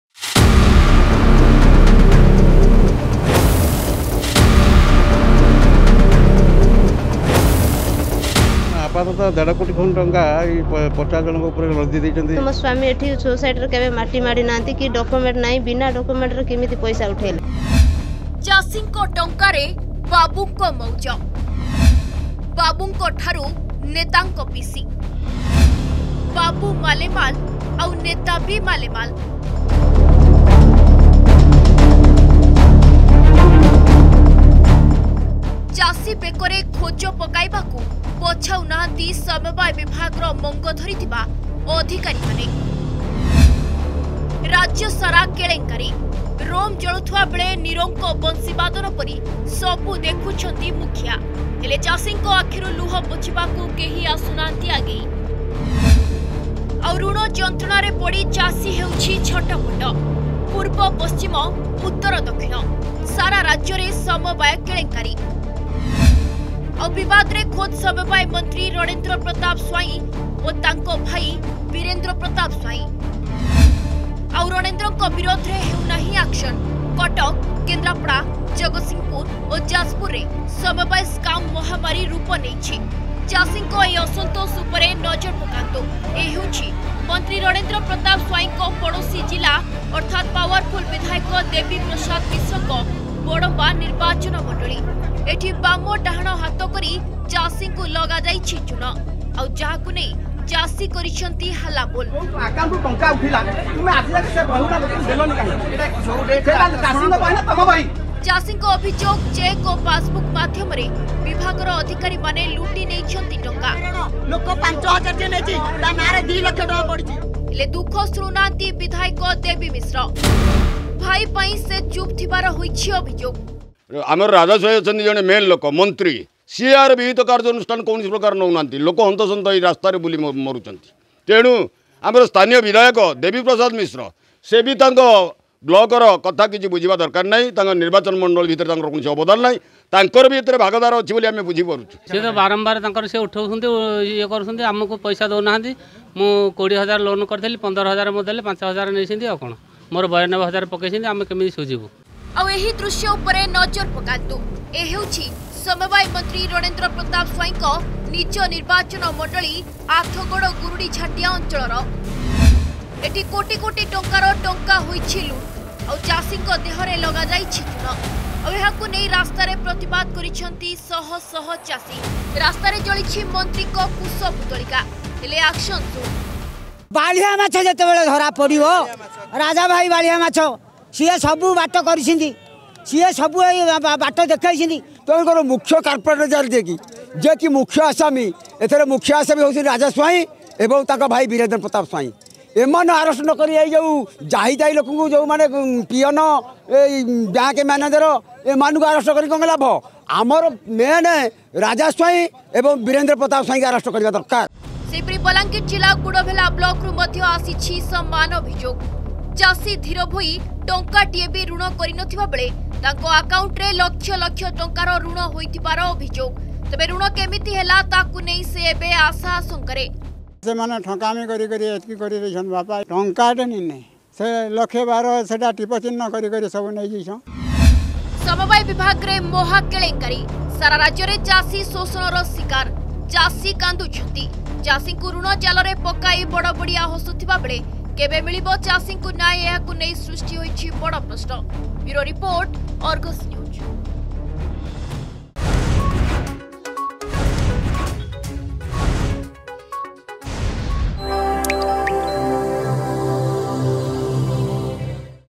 Cooperative Bank Scam Left Farmers Helpless in Balangir | ArgusNews
Cooperative Bank Scam left farmers in loan debt in Balangir alike Aathgad, Badamba. Farmers alleged MLA Ranendra Pratap Swain to be the main mastermind behind this scam.